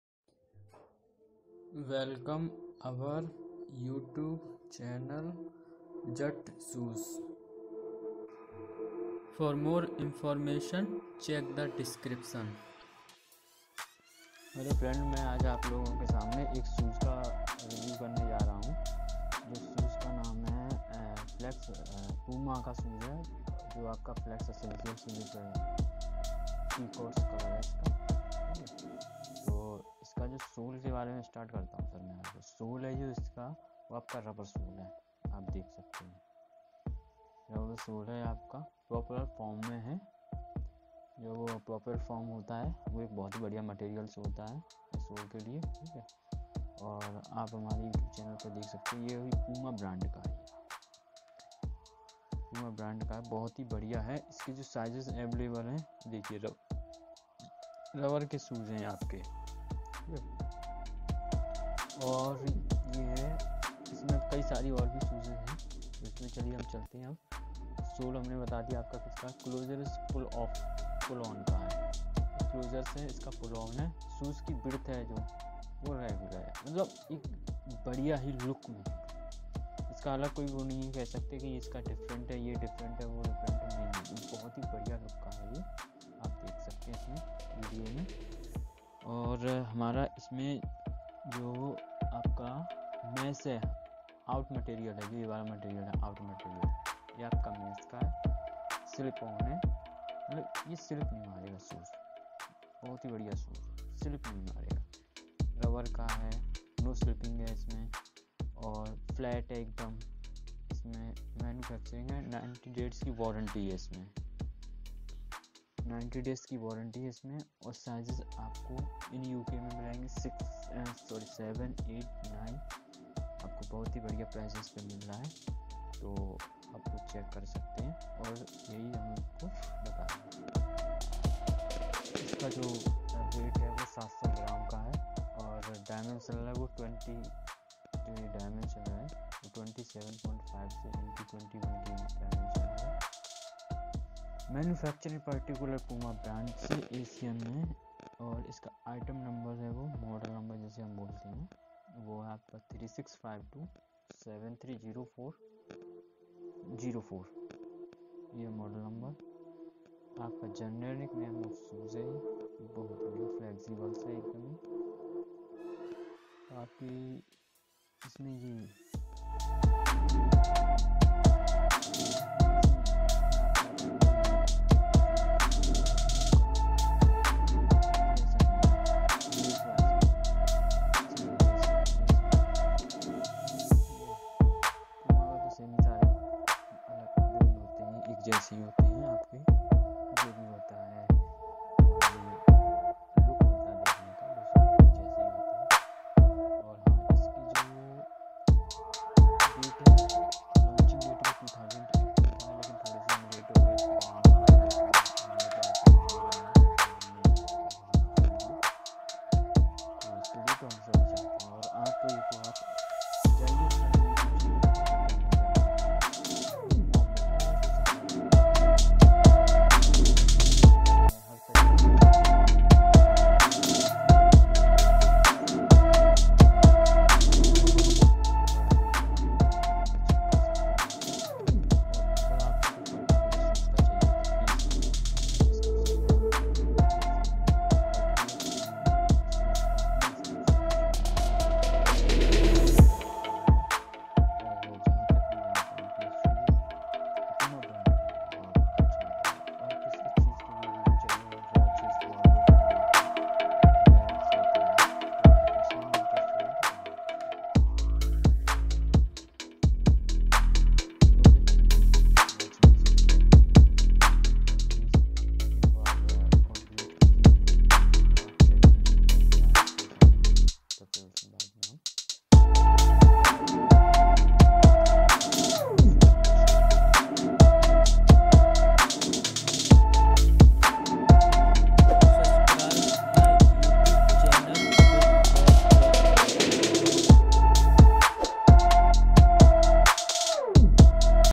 Puma Unisex-Adult Flex Essential Slip On Peacoat| by JatShoes
Description-
Sole: Rubber
Closure: Pull On
Shoe Width: Regular
Outer Material: Mesh
Closure Type: Slip On
Heel type: flats
Toe Style: Round Toe
Warranty Type: Manufacturer
Warranty Description: 90 days

Product details-
Package Dimensions ? : ? 27.5 x 21.5 x 10 cm; 700 Grams
Date First Available ? : ? 29 June 2018
Manufacturer ? : ? PUMA
ASIN ? : ? B07F3N9GYY
Item model number ? : ? 36527304_4
Department ? : ? Unisex-child
Manufacturer ? : ? PUMA
Item Weight ? : ? 700 g
Generic Name ? : ? Shoes
The grey Puma Flex essential slip on is from the running shoes collection from Puma. Comprising of stylish mesh upper and rubber sole this is lightweight, durable and comfortable, designed to perfect your performance.